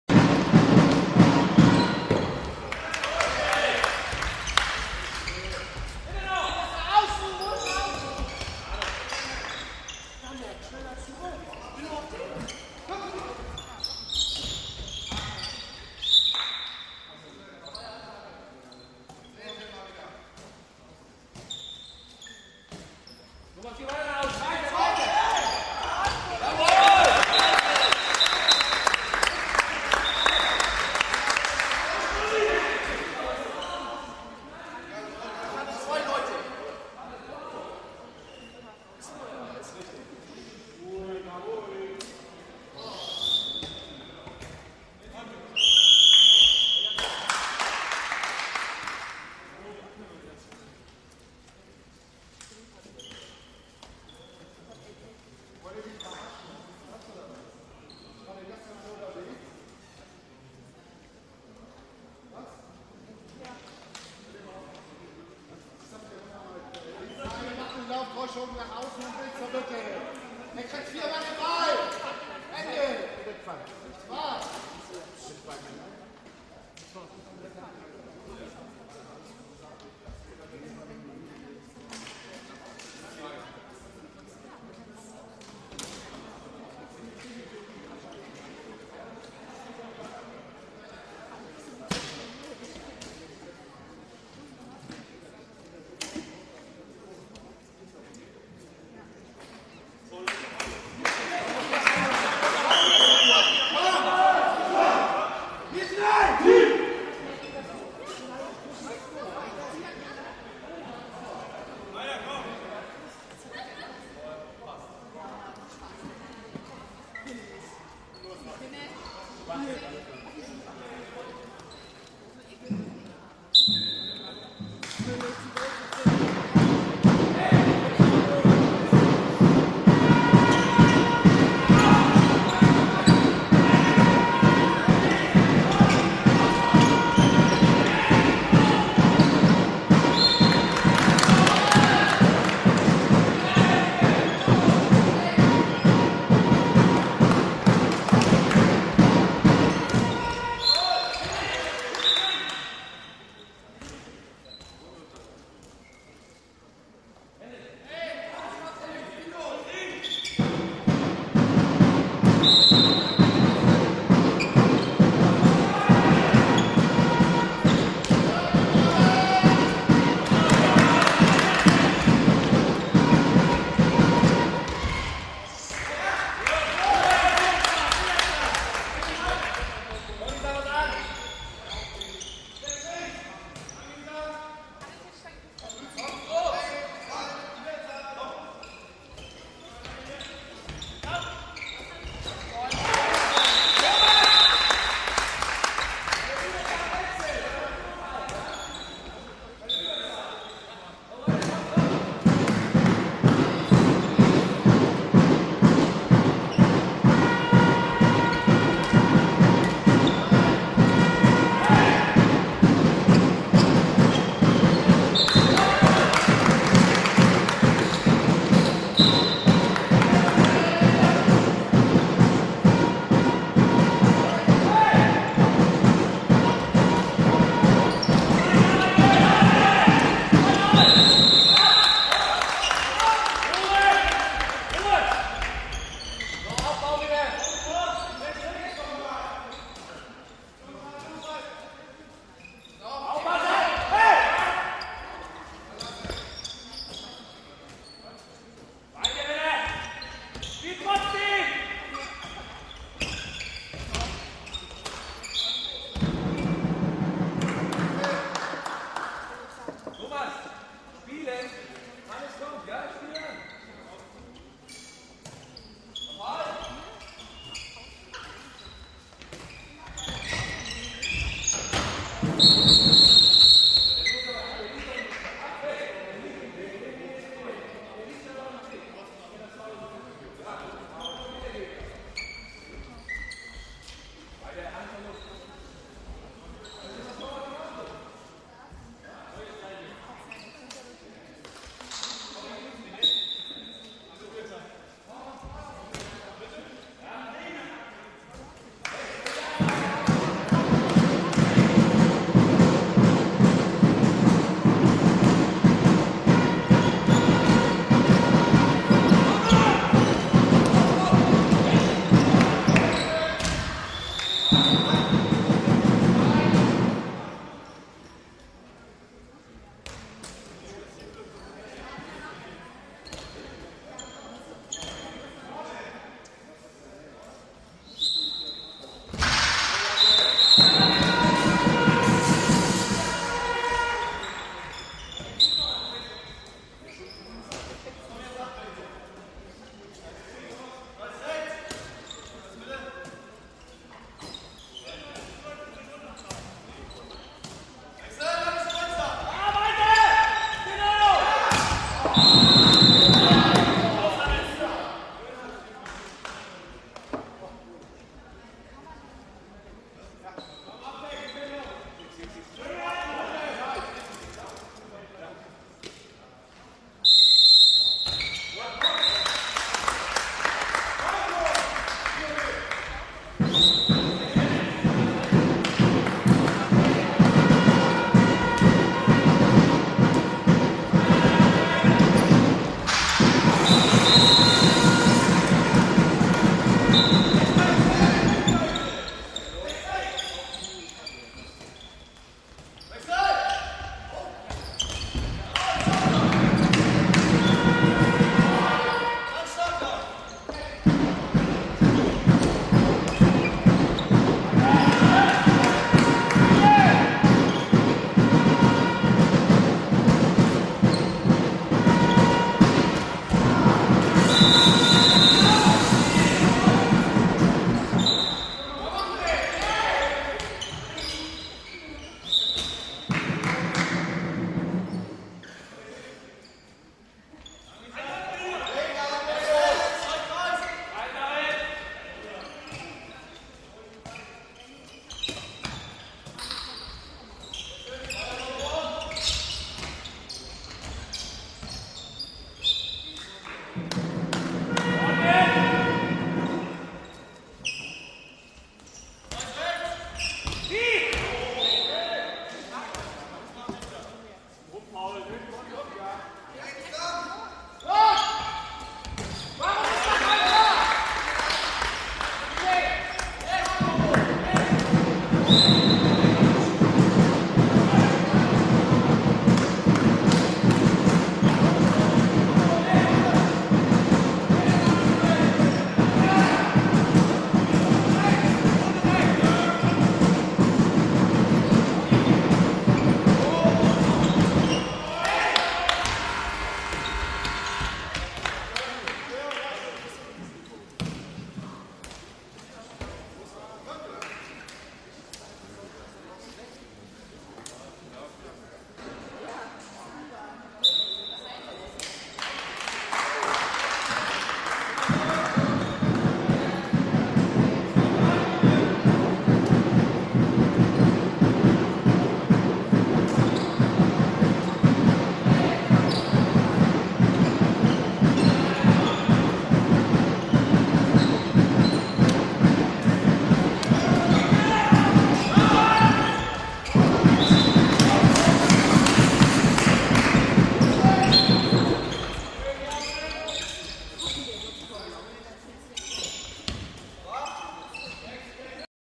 Sport inSchwerin 35s1 T 04
Handball Regionalliga mJA 27:45 SV Post Telekom Schwerin vs. HSG Schülp/Westerrönfeld